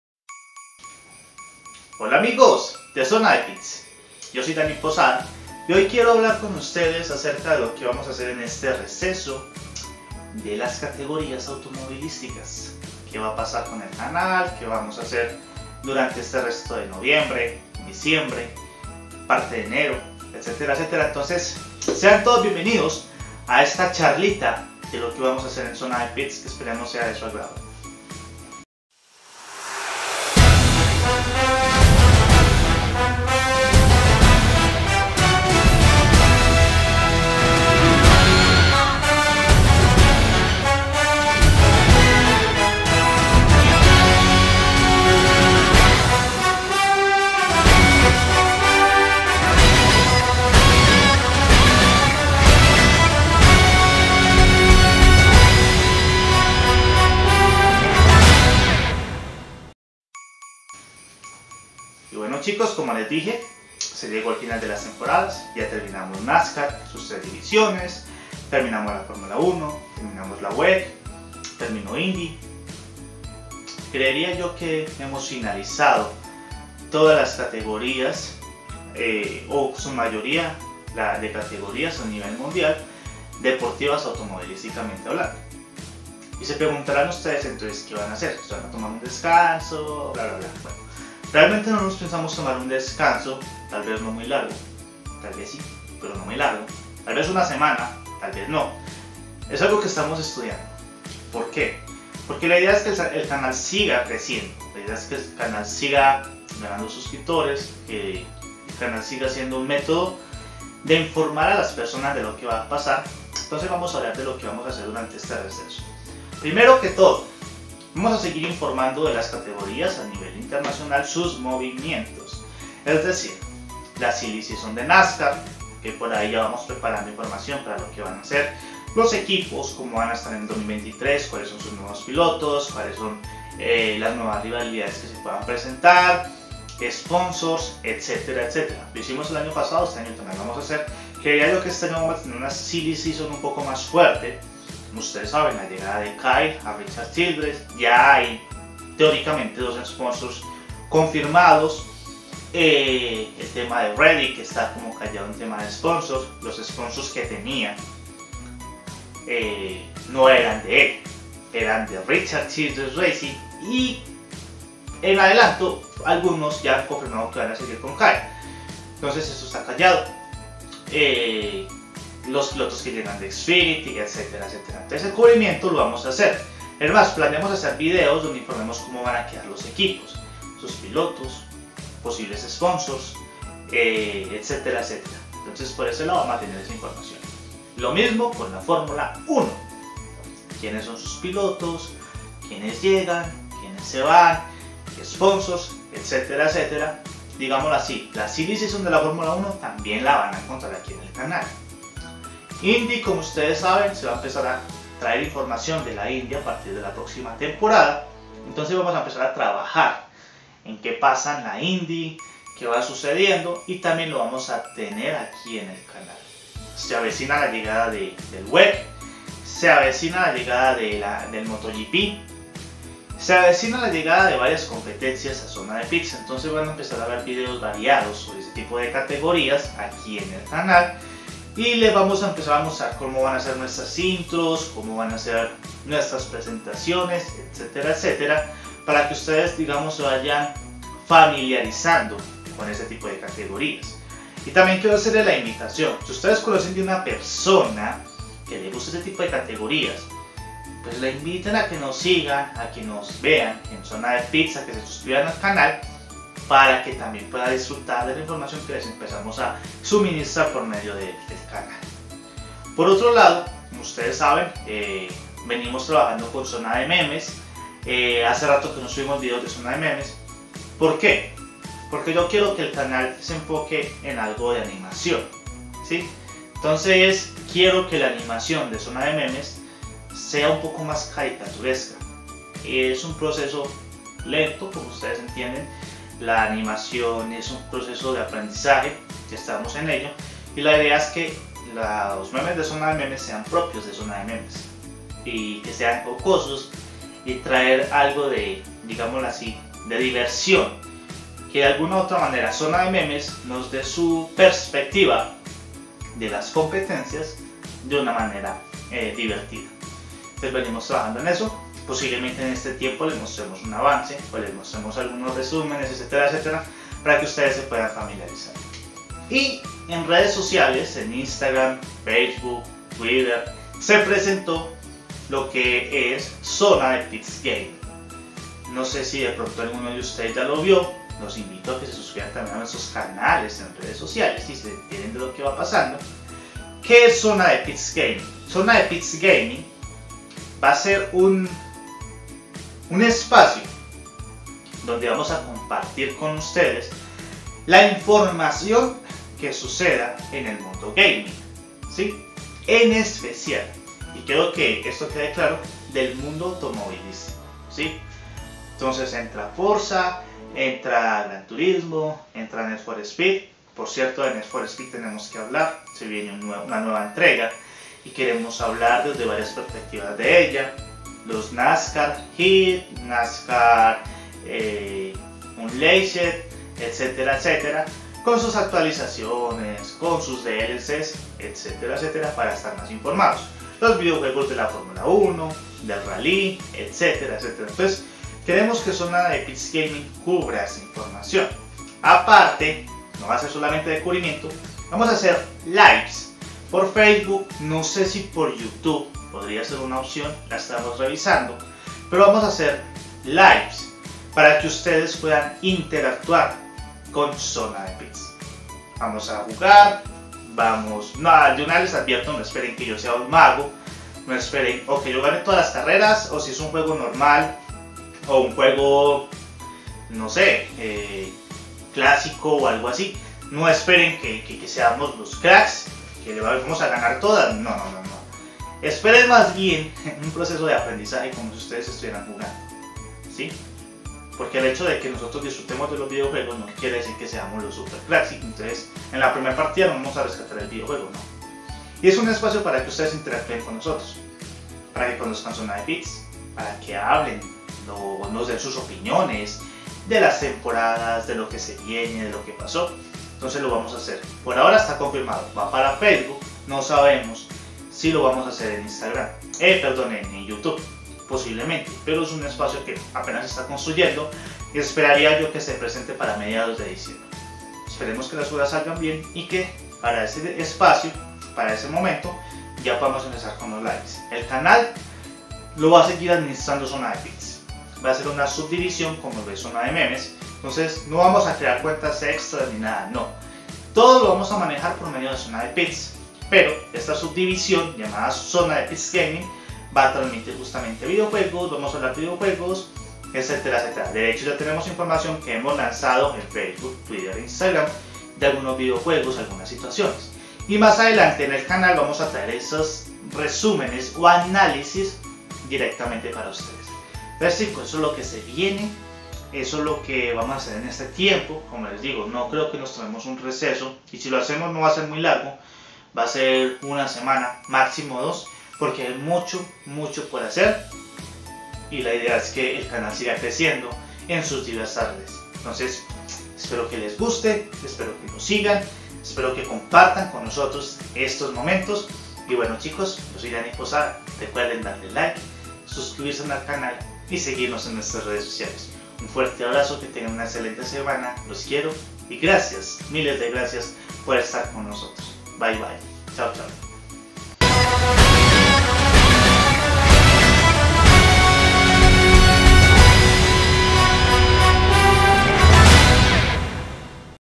LO QUE SE VIENE PARA ZONADEPITSS EN ESTA EPOCA
Hablemos de lo que haremos en Zonadepitss en ésta época navideña, Zonadememes y Zonadepitss Gaming, nuestro nuevo espacio para jugar e interactuar, los próximos videos que haremos y análisis.

Saludos.